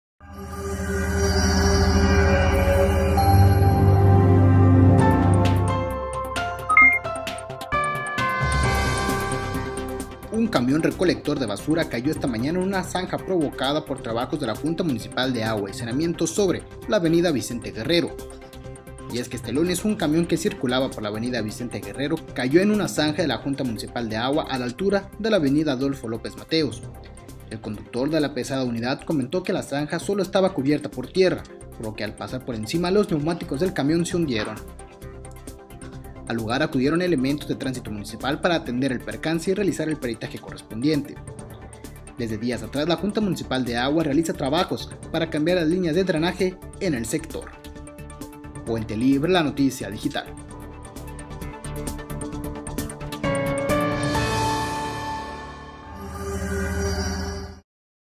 Cae camión en zanja de la JMAS sobre Vicente Guerrero
Un Camión recolector de basura cayó esta mañana en una provocada por trabajos de la Junta Municipal de Agua y Saneamiento sobre la avenida Vicente Guerrero.